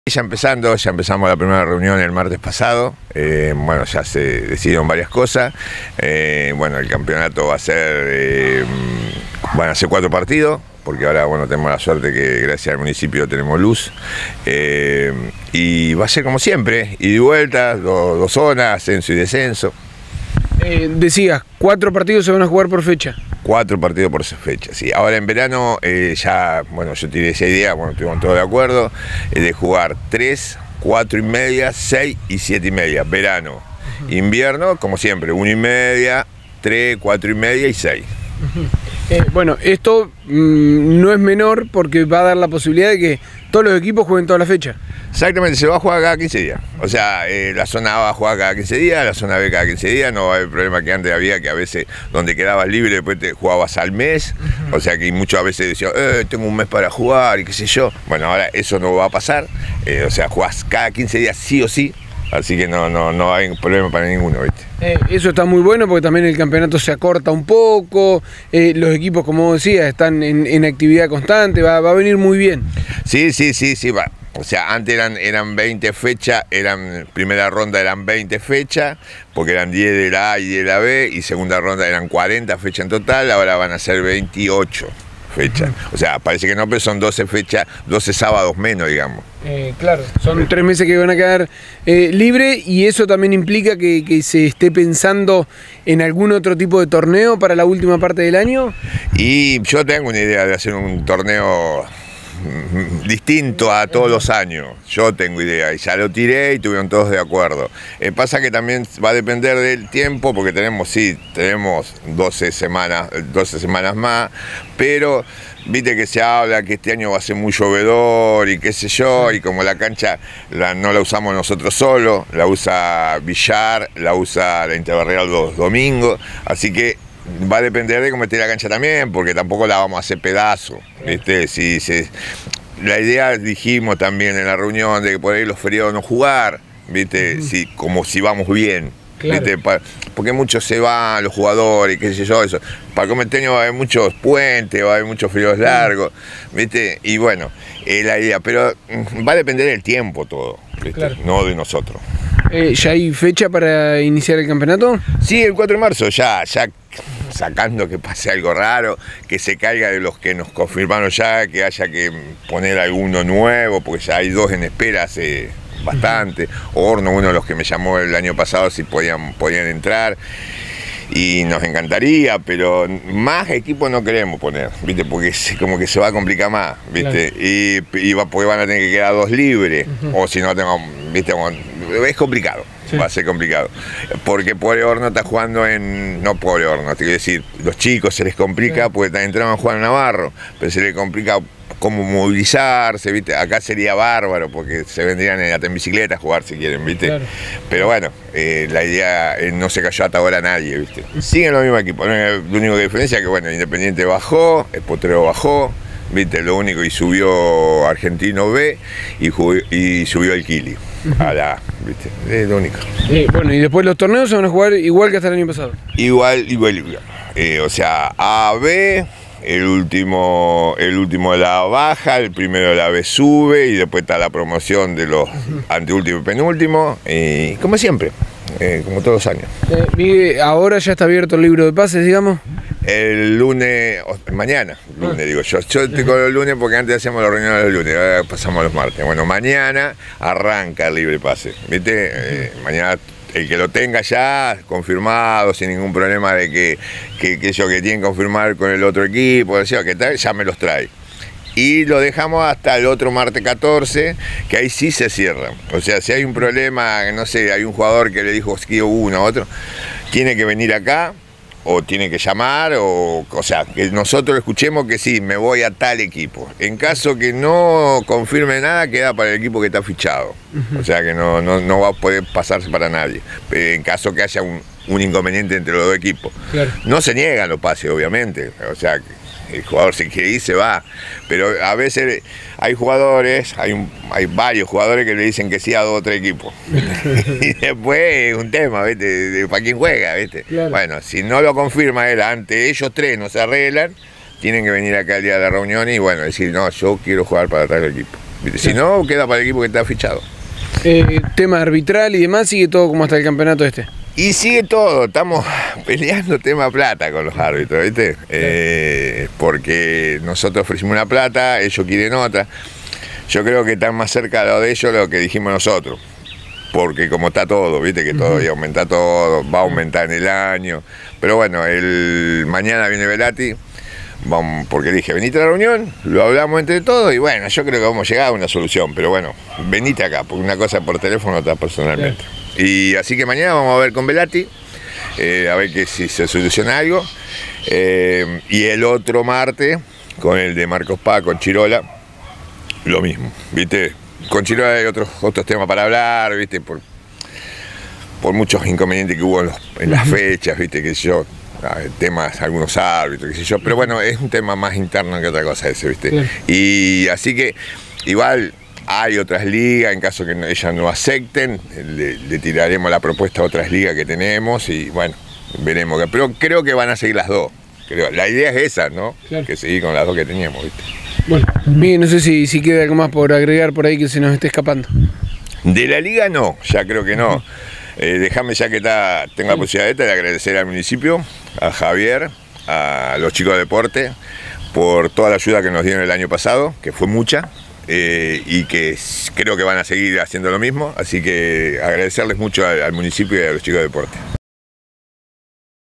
MIGUEL VILARIÑO- LIGA HERENSE
El 4 de marzo comienza el torneo de la Liga Herense de Fútbol.

Este año tendrá una modificación muy esperada: Se jugarán cuatro partidos por sábado, por lo que no quedarán equipos libres y todos jugarán cada 15 días.

Dialogamos con Miguel Vilariño quien nos dio detalles de la temporada que se viene.